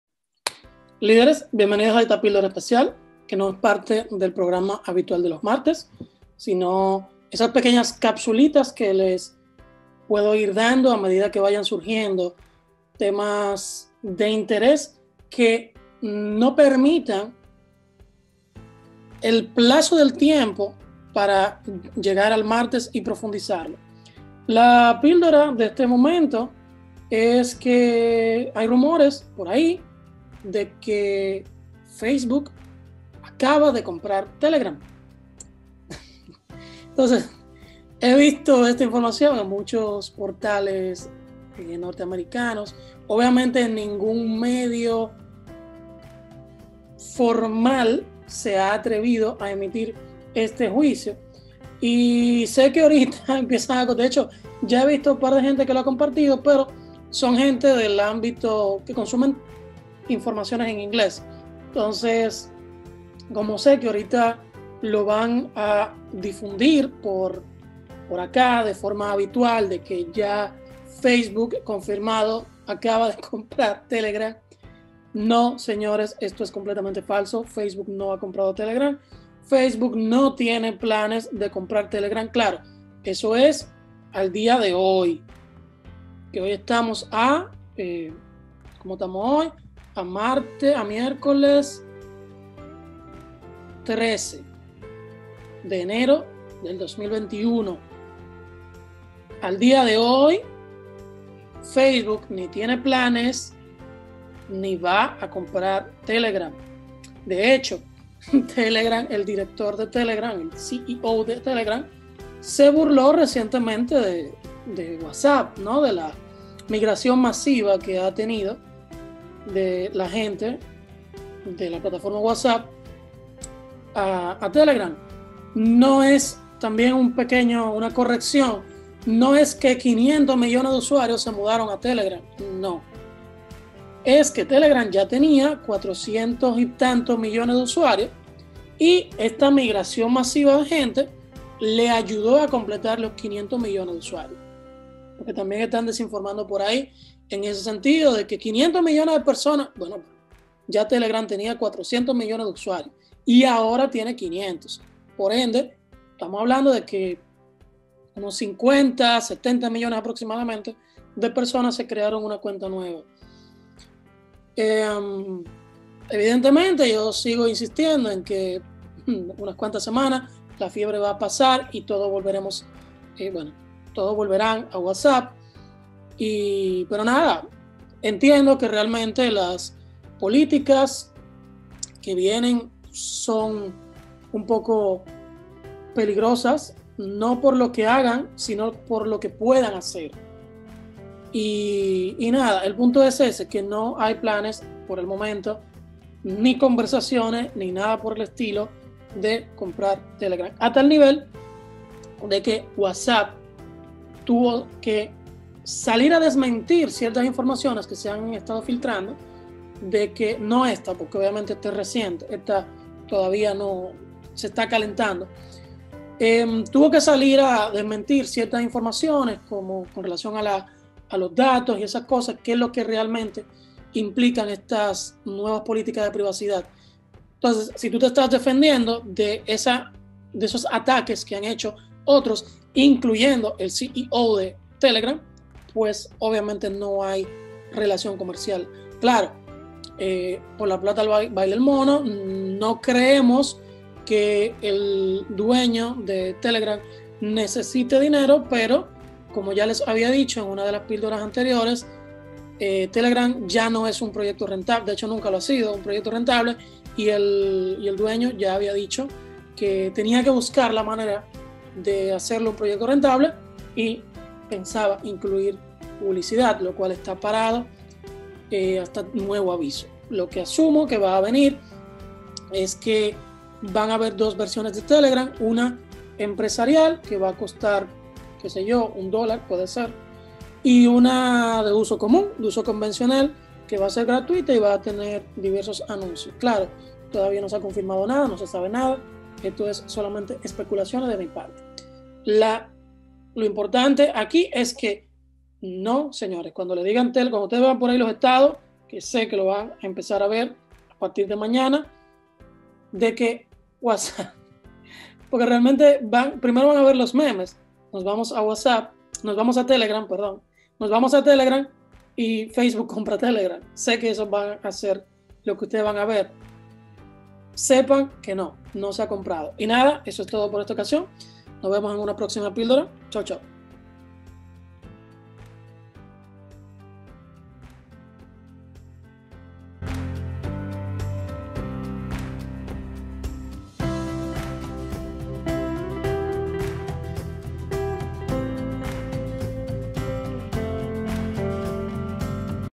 Facebook no ha comprado Telegram
Hay rumores completamente de que Telegram ha sido comprado, te cuento la falsedad de este buló.